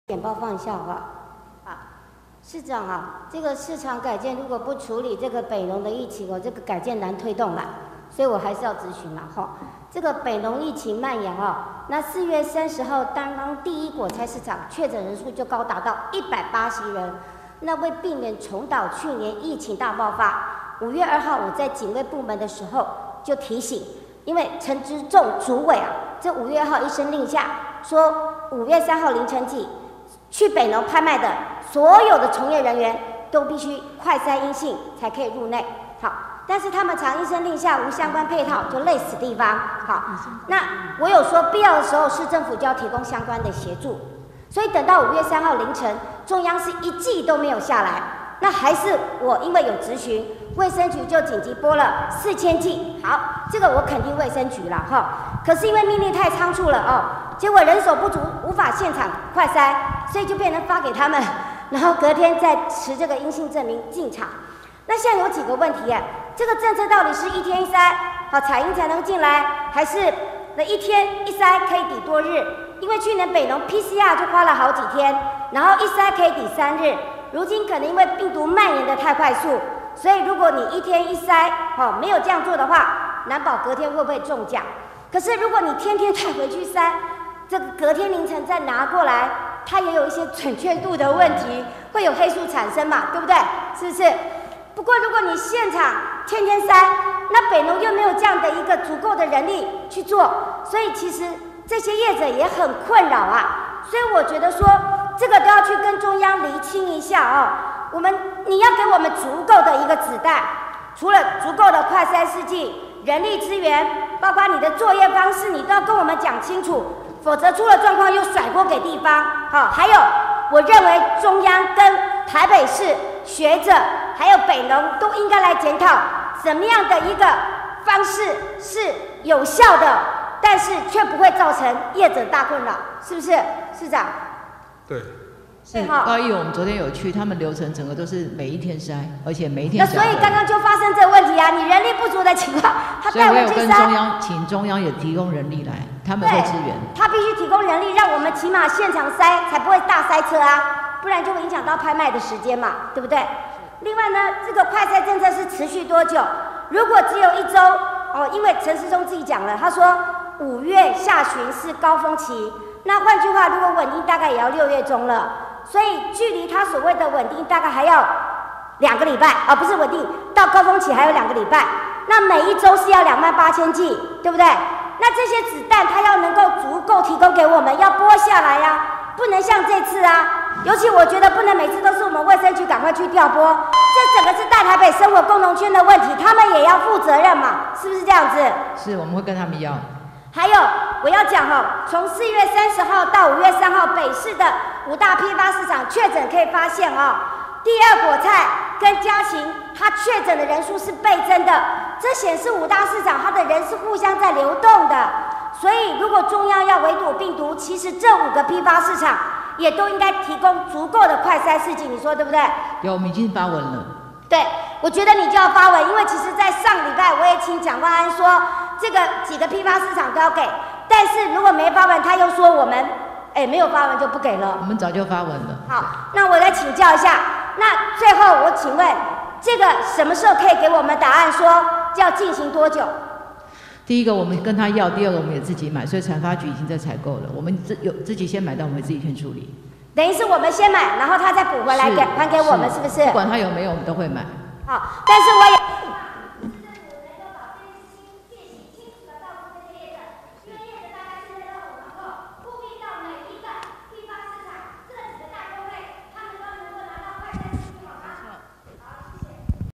1110505 第二次市長專案報告質詢-吉仲命令果菜市場快篩急就章，昭巖警衛質詢市府緊急撥試劑再要求市長釐清政策爭取資源，北市五大批發市場共同做防疫
北農疫情持續蔓延，郭昭巖議員為避免重蹈去年北農疫情大爆發事件，特別於111/5/5市長專案報告時再度提出質詢。郭昭巖向市長表示，4月30日單是第一果菜市場確診人數就高達181人!5/2她在警衛部門質詢時就提醒市府，農委會主委陳吉仲5/2一聲命令要求5/3凌晨起，凡進去北農拍賣所有從業人員必須「快篩陰性」才能入內。但中央常是一聲令下卻無相關配套累死地方，必要時市府要先提供快篩試劑給以利市場拍賣作業。結果等到5/3凌晨中央是一劑也沒撥下來，還是因她質詢，衛生局緊急調撥了4000劑給北農。郭昭巖就其快篩作業是如何落實執行且能有效圍堵病毒又不擾民下，要求市府和中央及學者和北農進行相關研議。也就該政策施行期多久、及中央市府應提供足夠快施試劑，與北市五大批發市場甚至環南市場都應有足夠快篩試劑以利有效防疫。市府回應已行文至中央爭取，且無論中央是否及時撥發，市府也會自行採購以因應現狀。

    郭昭巖表示，中央除了遲至5/3一劑也未撥下，也是到5/3中午才通知市府去領取，若非她警衛部門質詢衛生局因而緊急救援，中央的政策根本無法執行。但也因命令倉促，北農人手不足無法負荷現場大量快篩，改成發給他們回去篩檢，5/4凌晨再持陰性證明進場。這一來就產生諸多問題。包括該項政策是一天一篩，採陰才能進場，還是一篩可抵多日？去年北農專案PCR就花了好幾日，當時一篩抵3日，如今恐因病毒變異下蔓延速度太快，一天一篩或許難預料隔天會不會中獎。但如果天天篩是帶回去篩了隔天凌晨進場帶來，也有篩檢準確度問題及黑數產生。但現場篩人力恐不足耗費時間延誤拍賣，天天都篩大家也困擾。

郭昭巖要求市府應向中央釐清啊!而且若是一天一篩中央必須給足快篩試劑，及人力支援，作業方式指導與配合，否則出狀況又甩鍋地方!她認為中央應和市府及專家學者與北農共同研議準確度高又不擾民的防疫作為!她同時指出，阿中部長曾說這波疫情到5月下旬是高峰，故要穩定下來也是6月中。市府應確認這個快篩政策要持續多久？如果只有一周，那還有兩周才到他所謂的高峰期，每周要2萬8000劑，中央得準時提供，不能每次都是衛生局調撥或提供，畢竟這是大台北生活共同圈的重要批發市場，從業人員來自不只台北新北更有全國各地。

    郭昭巖並就4/30和5/3北市五大批發市場確診人數發現第二果菜市場和家禽臨時批發市場確診人數增加倍數很快，分別是近3倍和4倍，這可以顯示五大批發市場的人常有彼此互相流動的現象!她認為中央若要圍堵疫病毒就應該五個批發市場、甚至環南市場都要提供快篩試劑才是，故她除在上周拜託蔣萬安委員向中央爭取北市批發市場快篩試劑供給，也要市府盡速行文到中央做爭取，同時要求這些疫苗務必及時送到五大批發市場和這幾個公會業者手上。

市府表示，已行文至中央也會自行採購疫苗以因應下來的相關防疫作為，也會將疫苗送到相關作業人員手上。